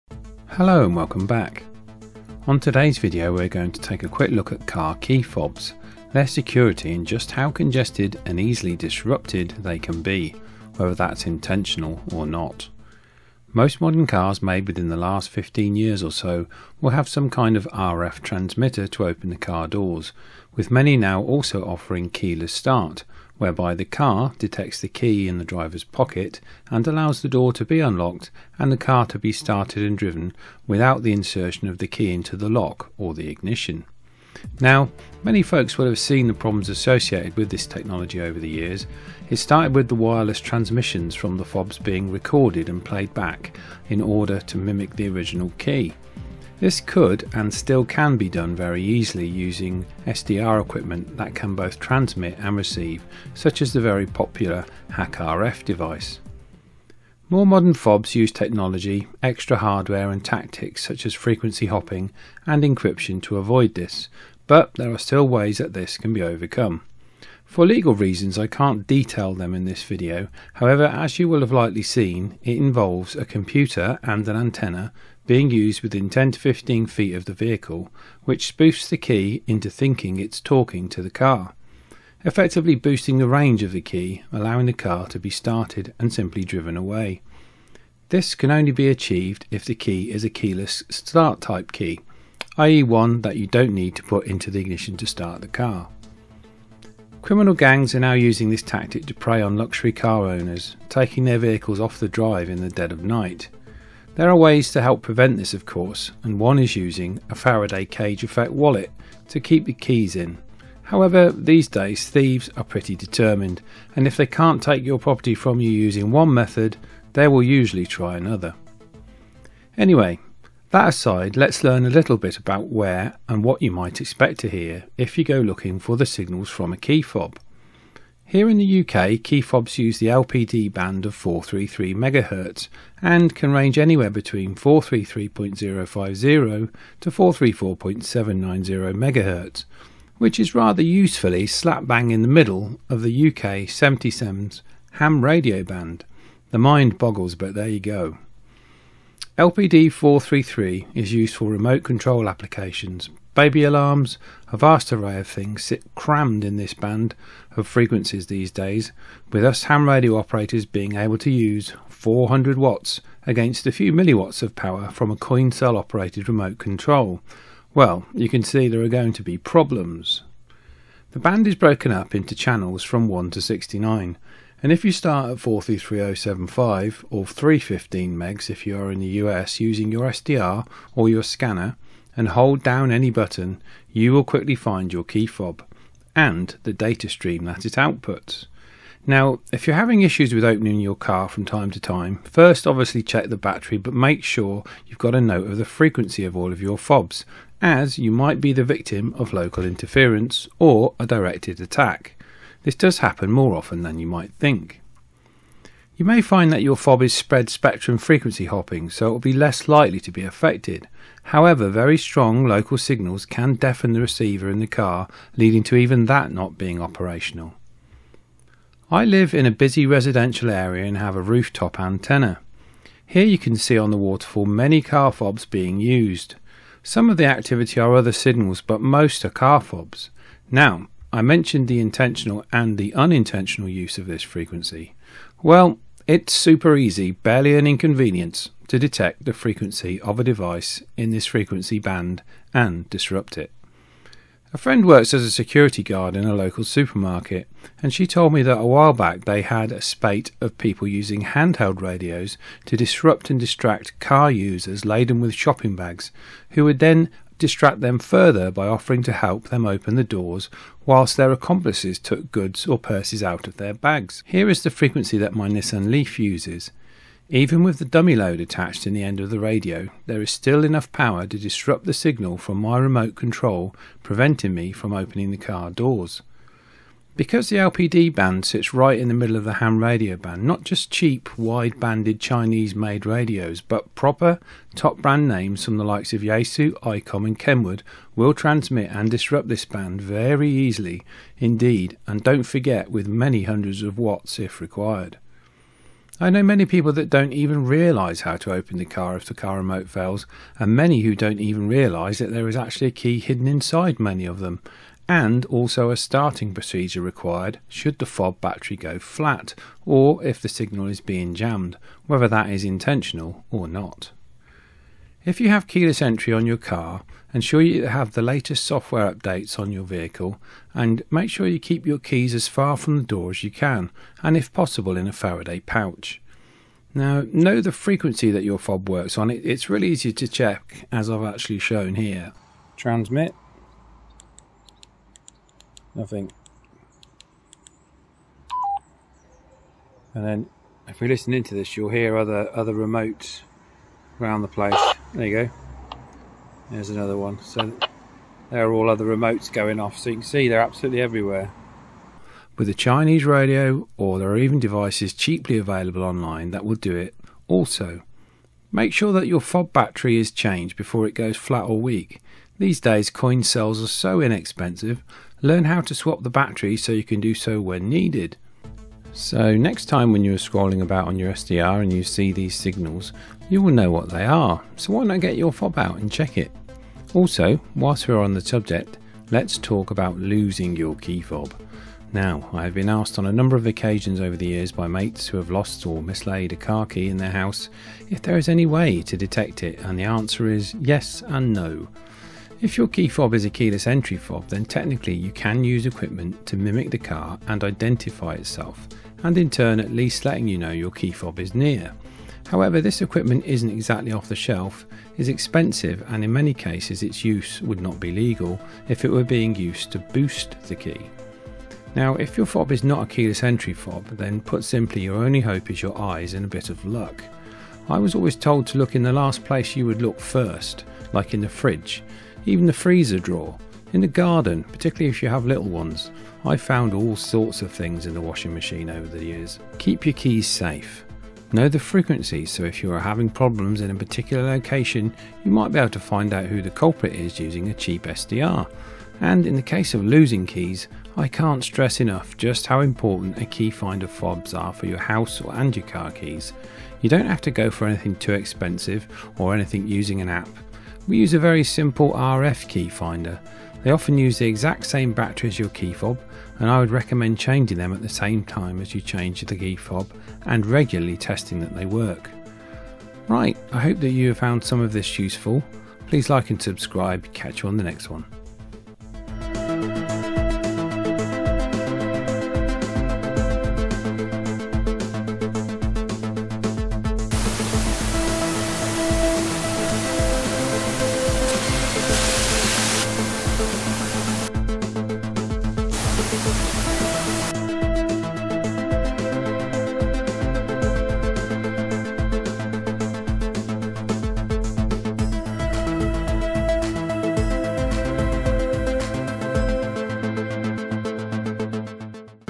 IS YOUR CAR KEY BEING JAMMED? AND THE EASY WAY TO FIND IT IF YOU LOSE IT.
On this video we have a brief look at the humble car key, great when it works but a nightmare when it doesnt and even worse if you lose it, check this video out to help ensure trouble free motoring.